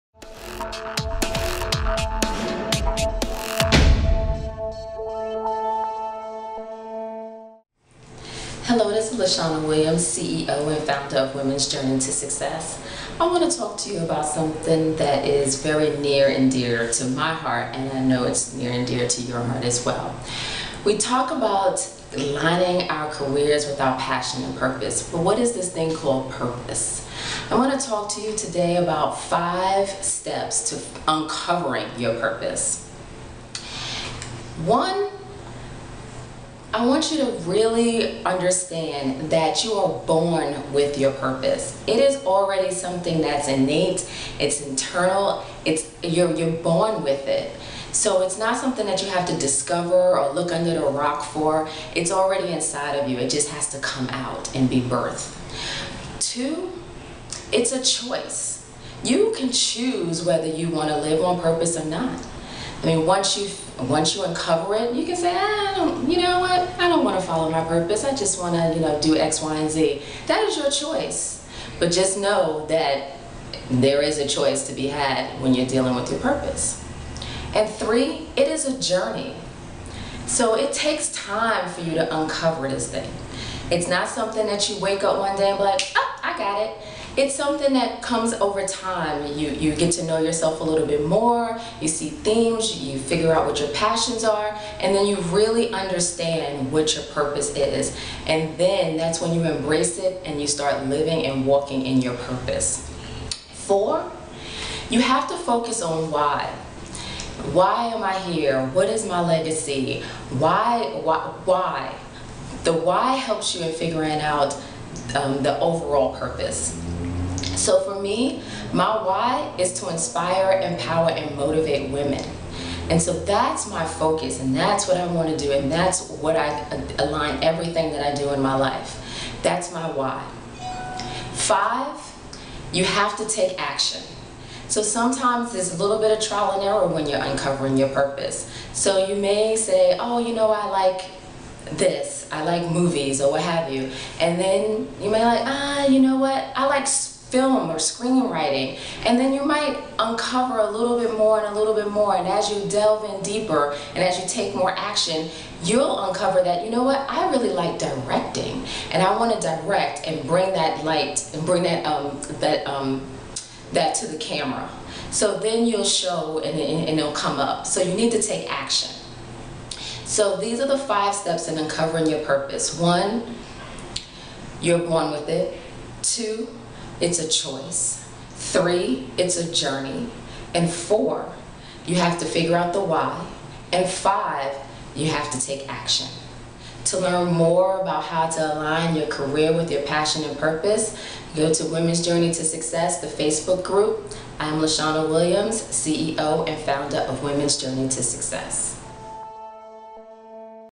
5 Steps to Uncover Your Purpose
Your purpose answers the question "why am I here". It also guides your life's personal and professional path. This video will help you unlock you passion and uncover your purpose so you can live your best life now.